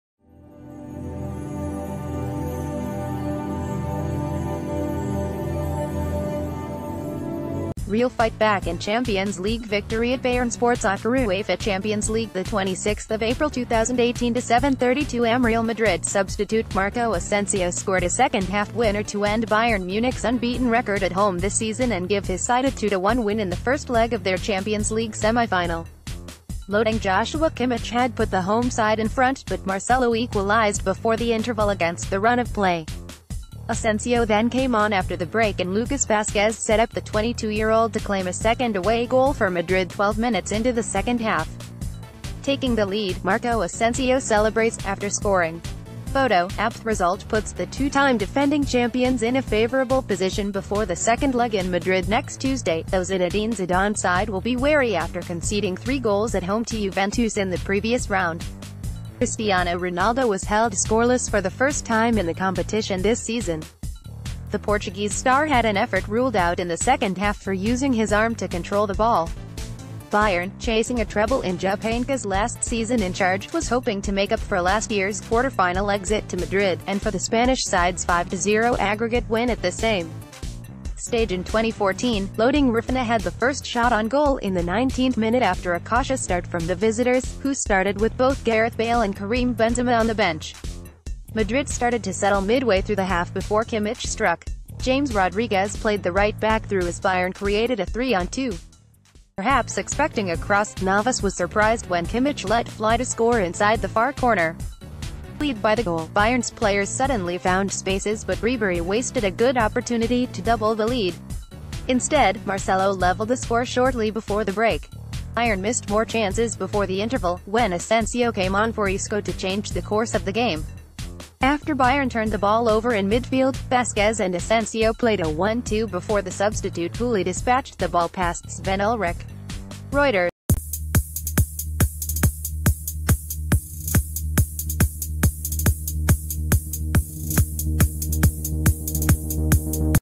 Breaking News | UEFA Champions league: Real Madid fight back against Bayern
Real fight back in Champions League victory at BayernSportSoccerUEFA Champions League26 April 2018 — 7:32amReal Madrid substitute Marco Asensio scored a second-half winner to end Bayern Munich's unbeaten record at home this season and give his side a 2-1 win in the first leg of their Champions League semi-final. LoadingJoshua Kimmich had put the home side in front, but Marcelo equalised before the interval against the run of play. Asensio then came on after the break and Lukas Vazquez set up the...